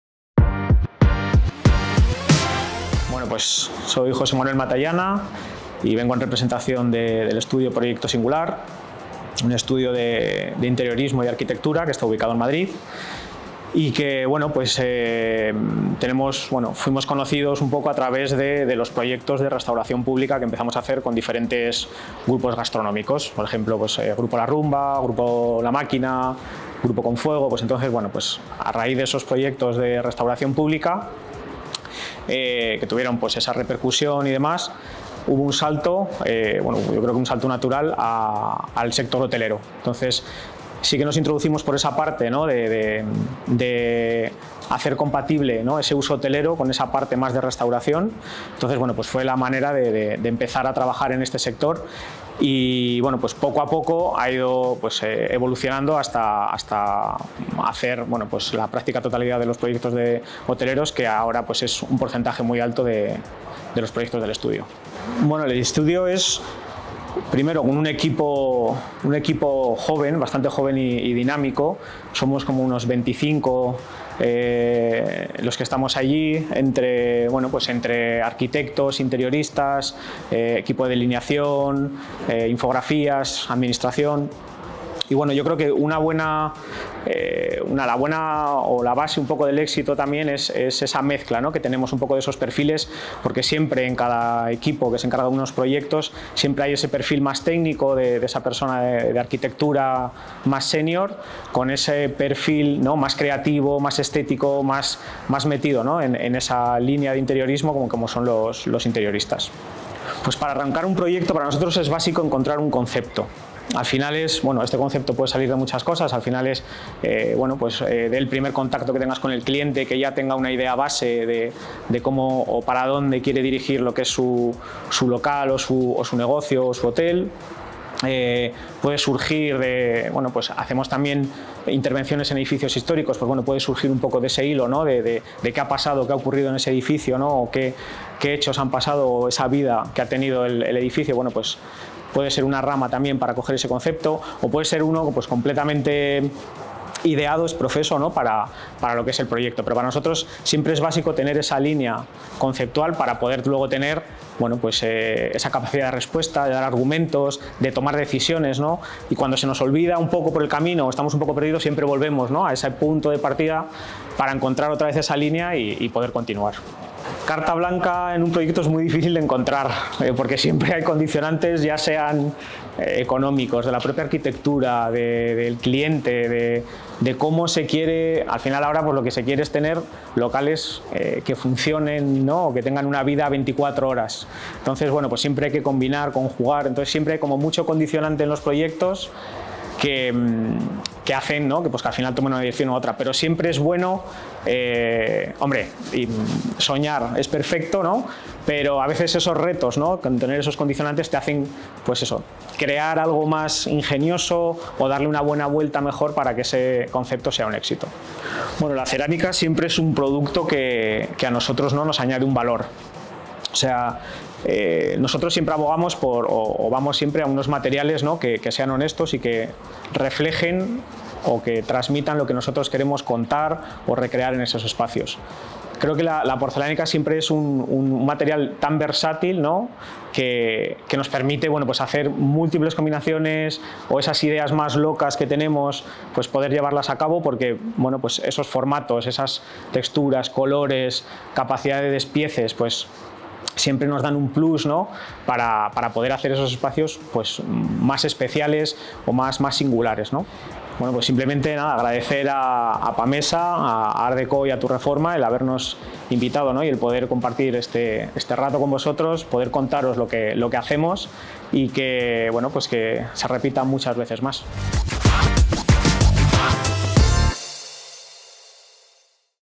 ARQ/DECO - Entrevista a Jose Manuel Matallana de Proyecto Singular
Durante esta entrevista, tenemos la oportunidad de comentar con Jose Manuel Matallana, de Proyecto Singular. En una icónica velada, nos acompaña en las instalaciones de Pamesa para celebrar la pasarela de tendencias ARQ/DECO 2024.